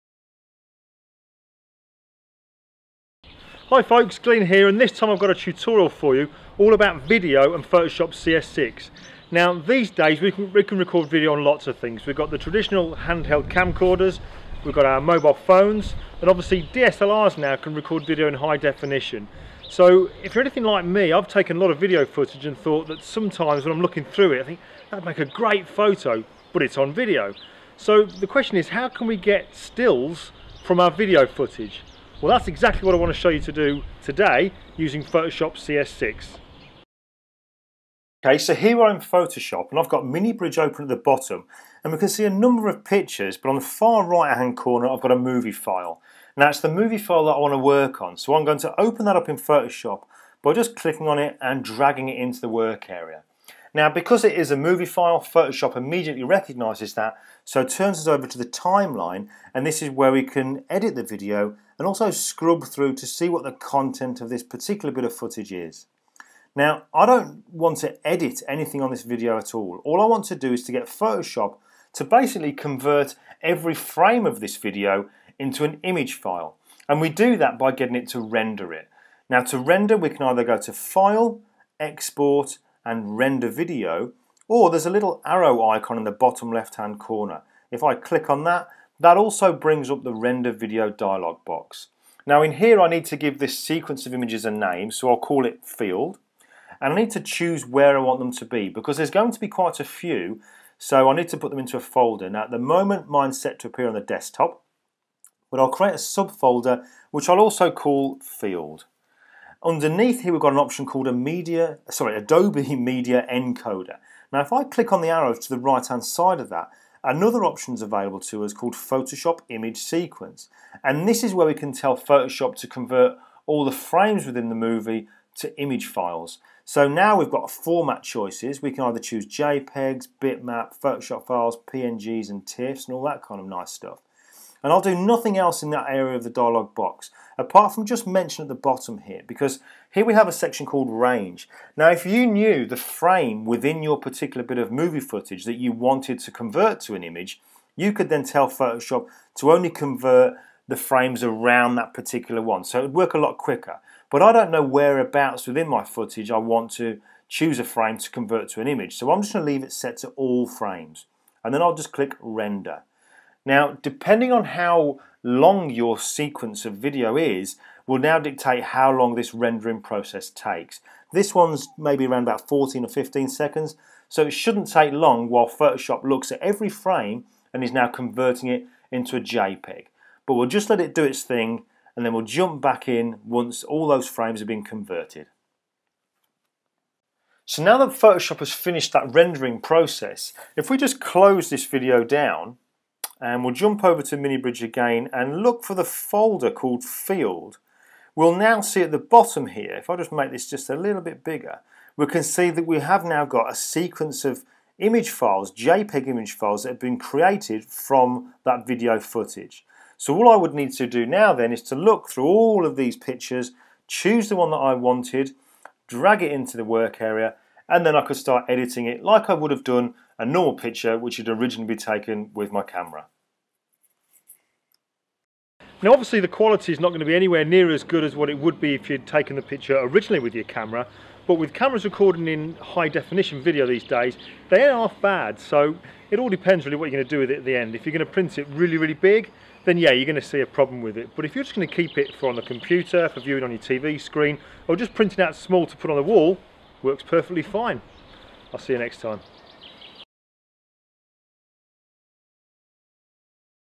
Turning Video into Photos with Photoshop CS6
Short tutorial showing how to convert video footage into image files that you can then edit, print etc... using Photoshop CS6